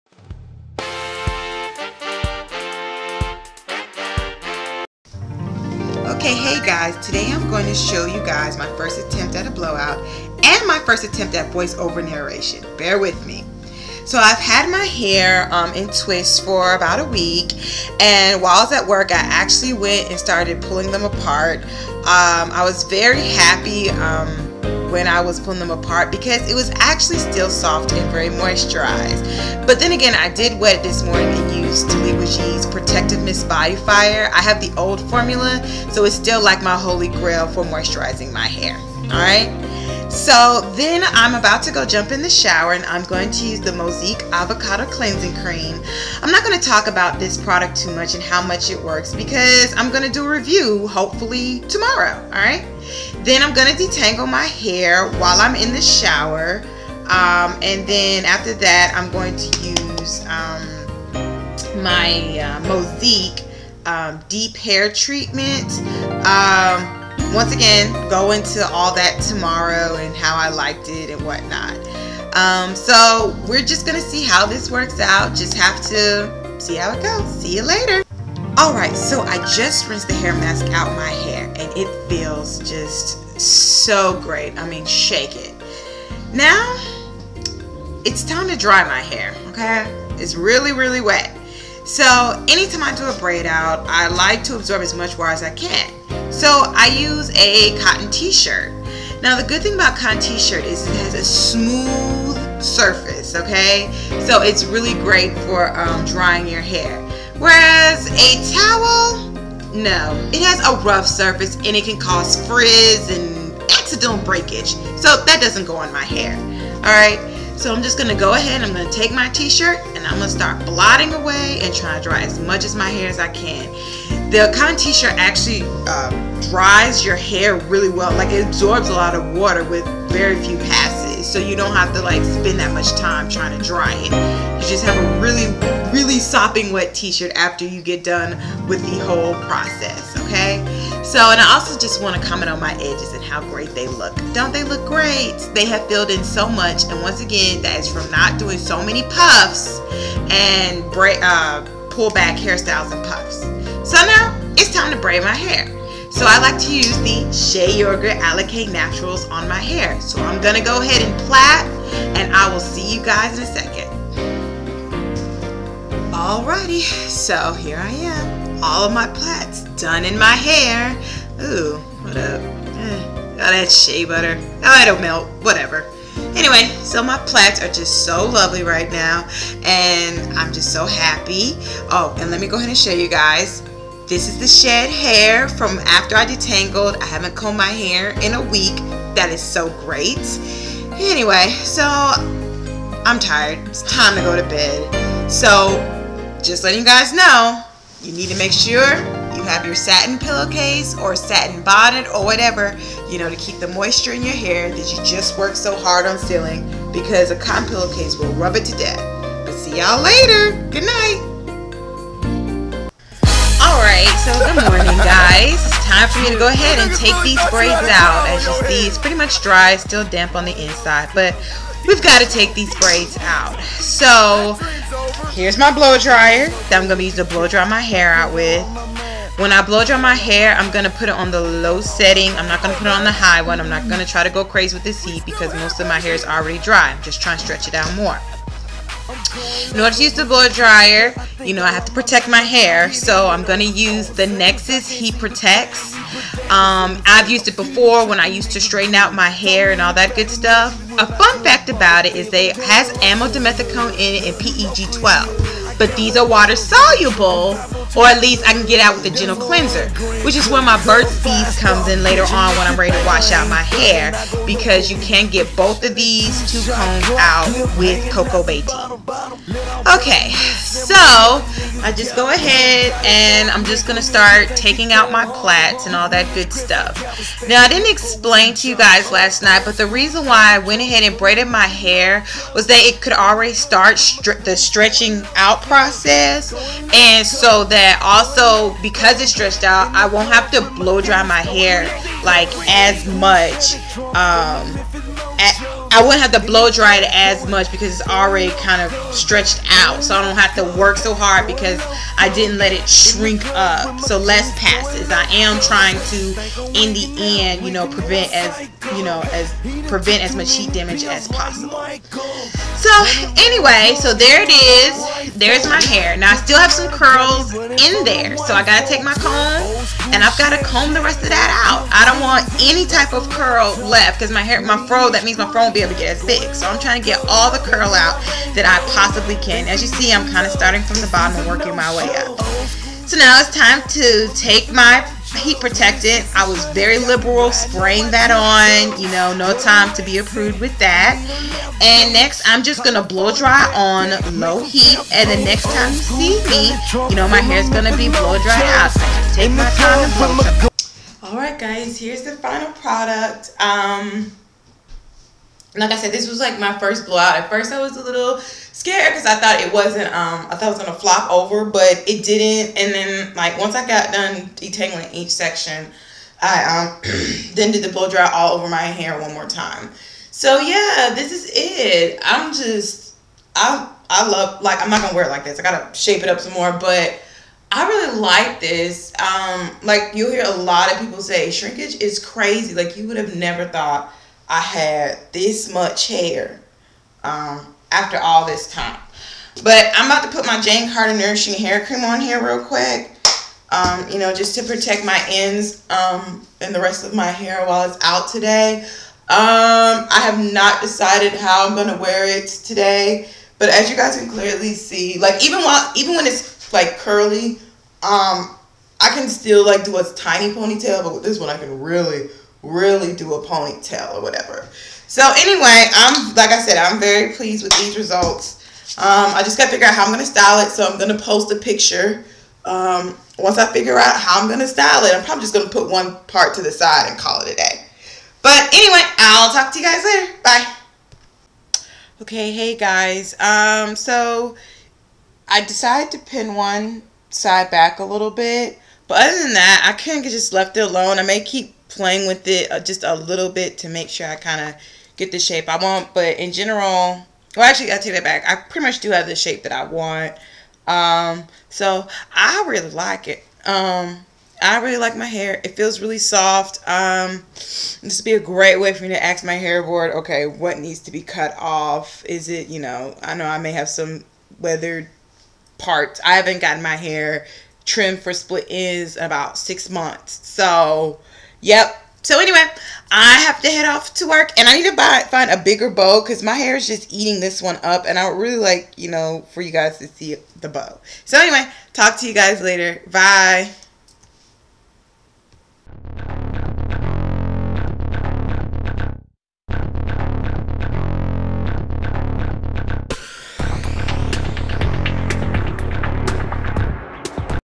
My First Blow Out
So here is how I blew out my hair. Once again sorry about the video quality (yellow tinge) I hope to just get a different camera soon.